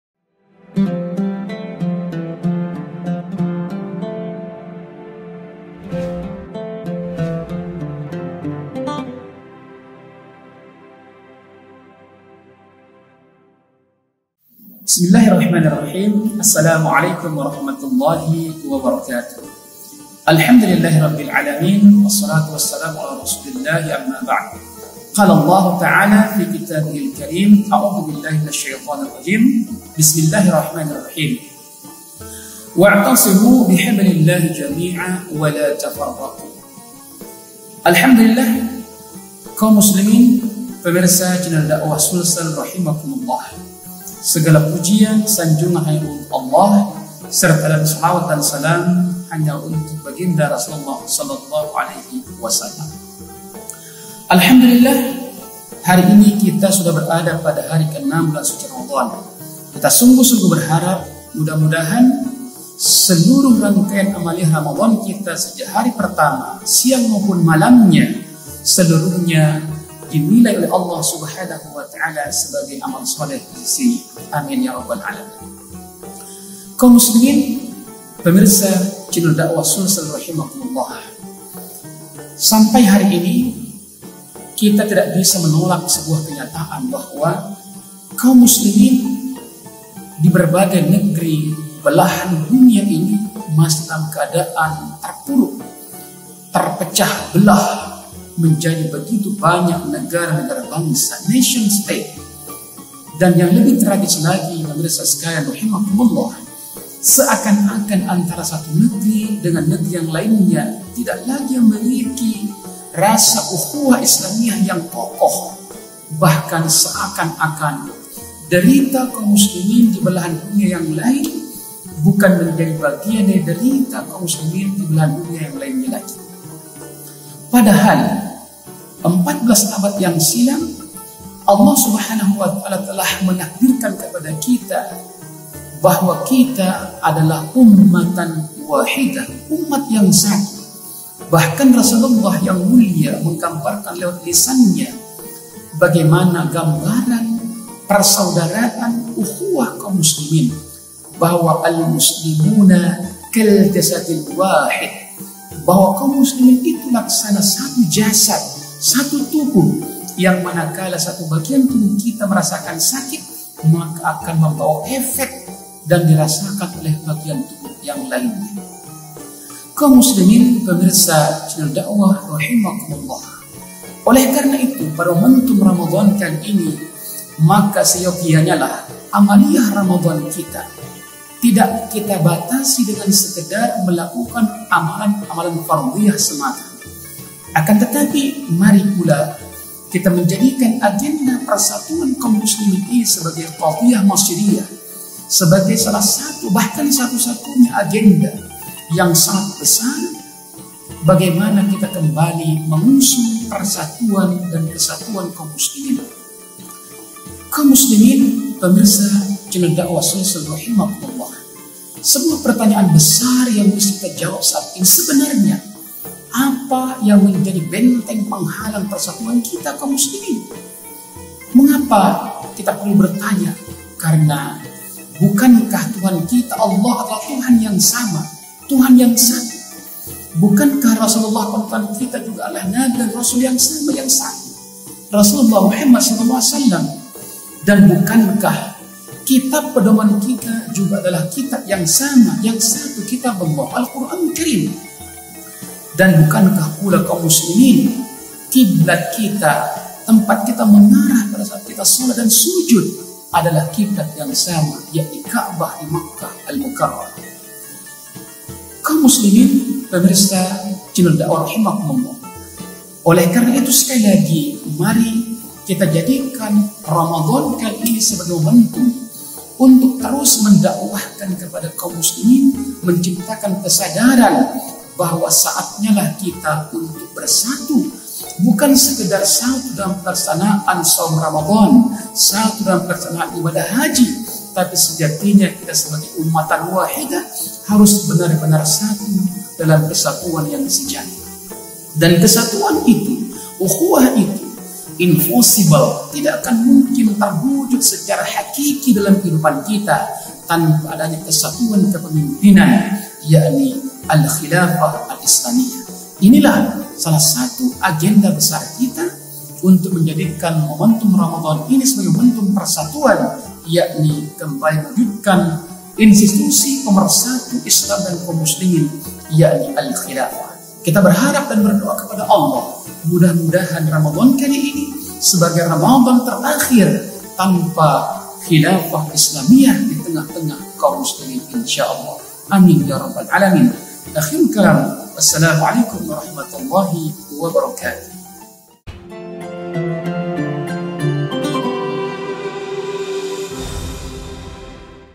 Kajian Tausiyyah Malam #6 : "Ramadhan Bulan Persatuan"
Oleh Ust. Muhammad Ali Wardana
Saat ini kita melihat kaum muslim berada dalam keadaan terpecah
belah yakni dalam bentuk nation state. Padahal Rasulullah
Shollallahu Alayhi Wasallam sudah menegaskan bahwa umat islam ini
layaknya sebuah tubuh yang apa bila satu bagian sakit maka yang
lain pun turut merasakannya